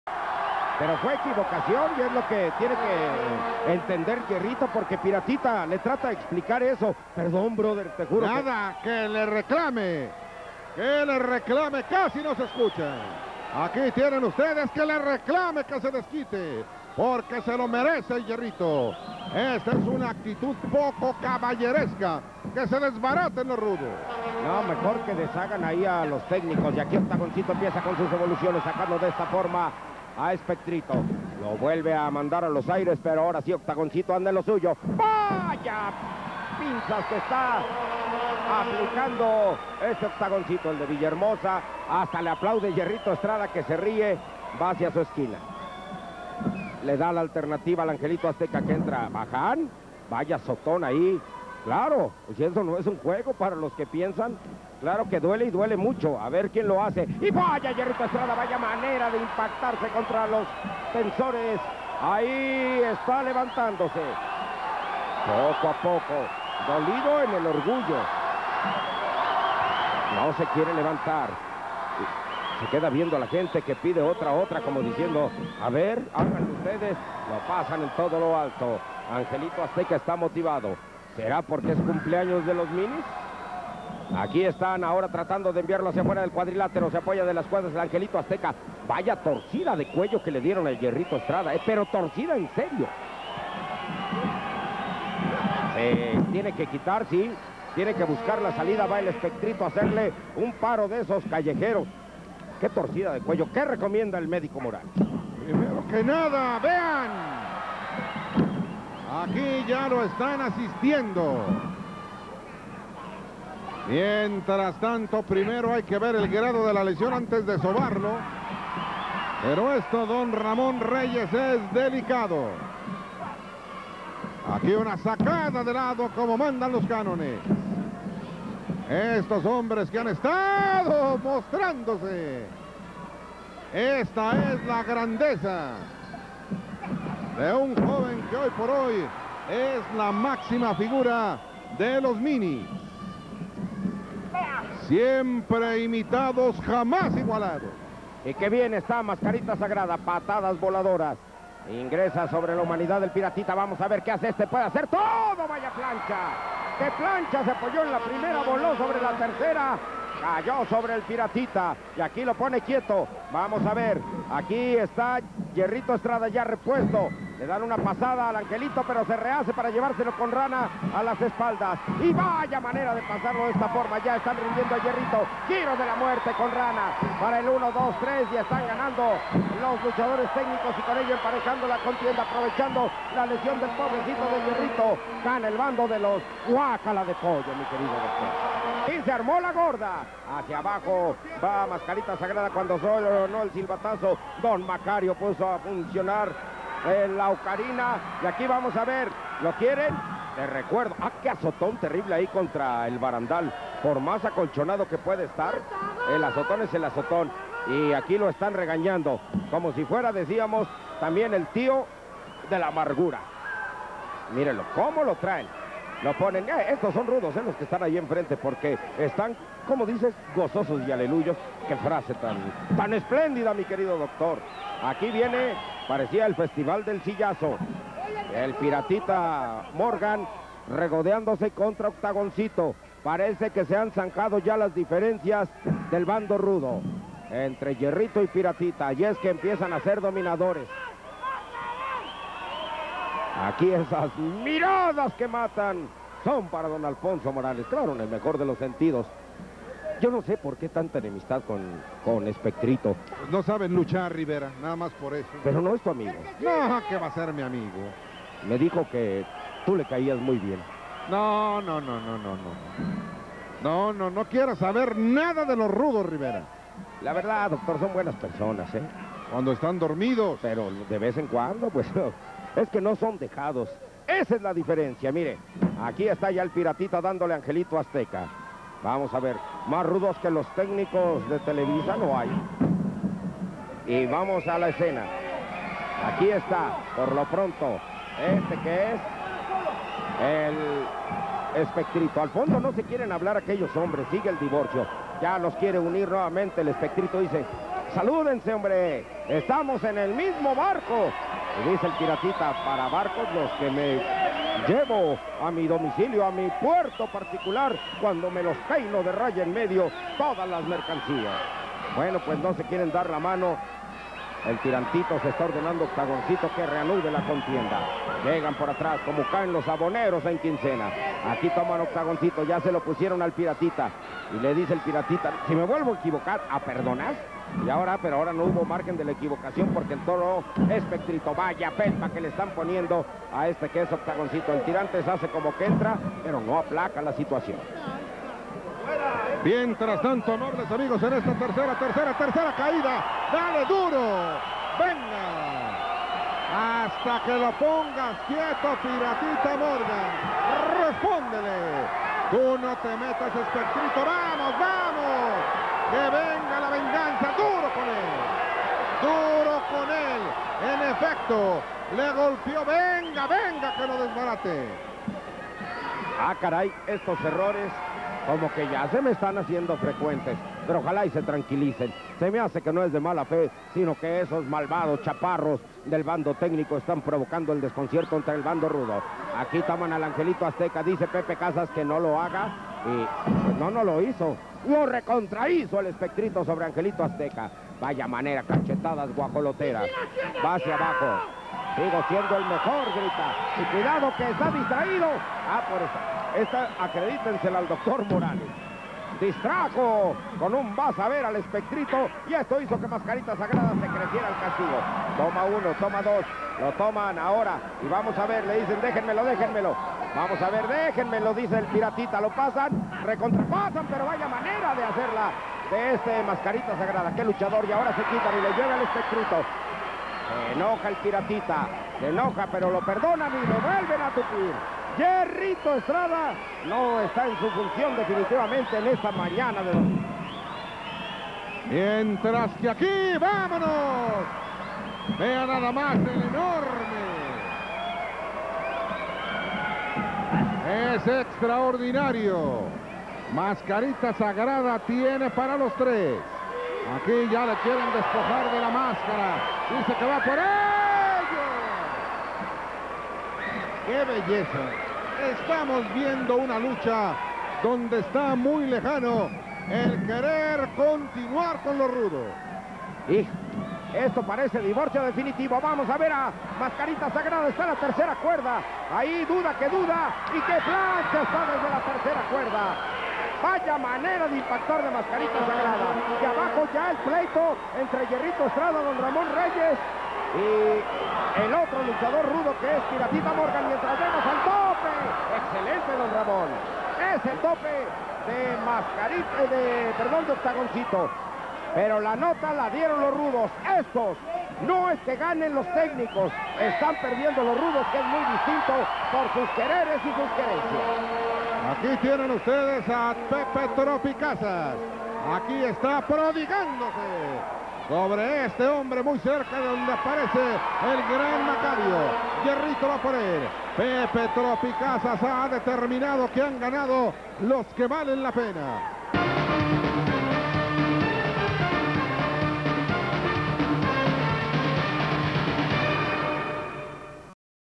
Angelito Azteca/Mascarita Sagrada/Octagoncito vs Espectrito/Jerrito/Piratita Morgan - AAA 9/24/93
Angelito Azteca/Mascarita Sagrada/Octagoncito vs Espectrito/Jerrito Estrada/Piratita Morgan - AAA 9/24/93 (Part 2)